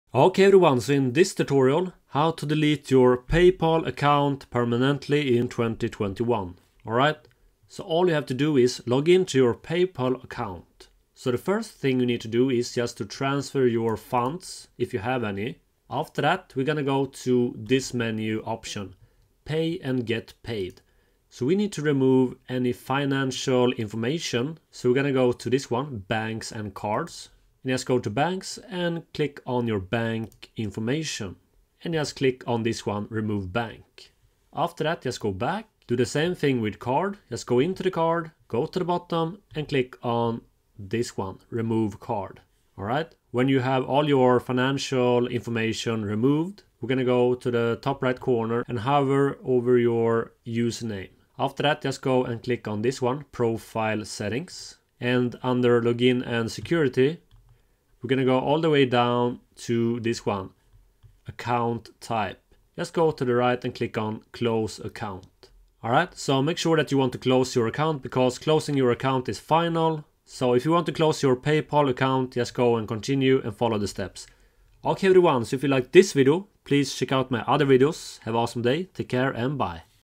How to Delete PayPal Account 2021
How to delete and close your Paypal account fast and easy in 2021.

Before you close your PayPal account permanently, make sure to remove any financial information (card and bank account).

Then you need to withdraw money from your PayPal account to your bank account.

All your pending payments will be automatically canceled.

And to be able to manually close your account, you must be logged into the PayPal website.

This action is final and can not be reversed.